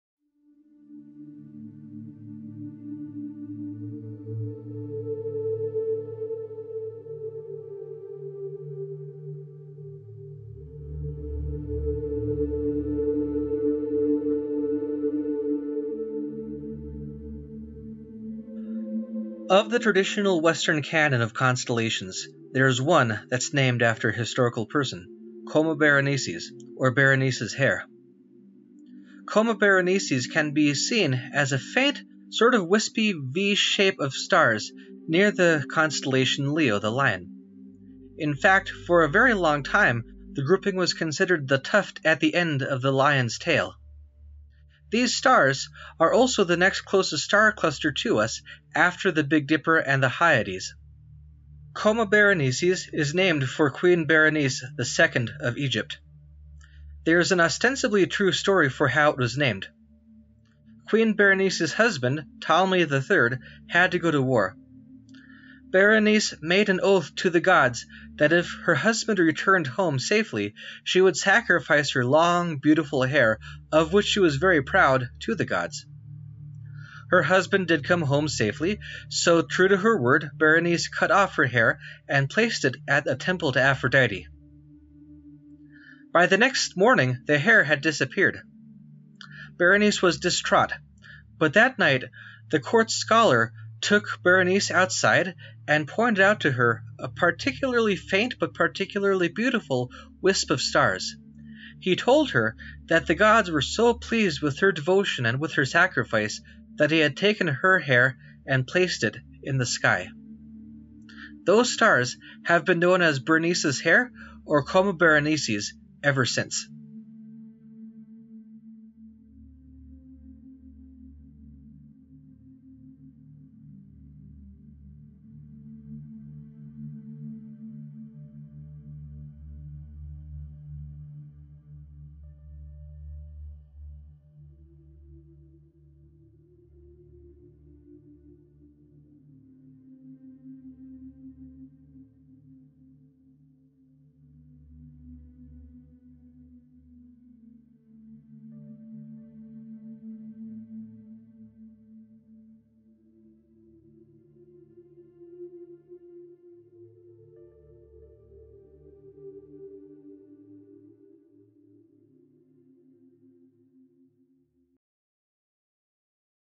Sagas in the Sky | Coma Berenices
Sagas in the Sky explores some of the stories behind the stars in the night sky. In this episode, we look at the story of Coma Berenices.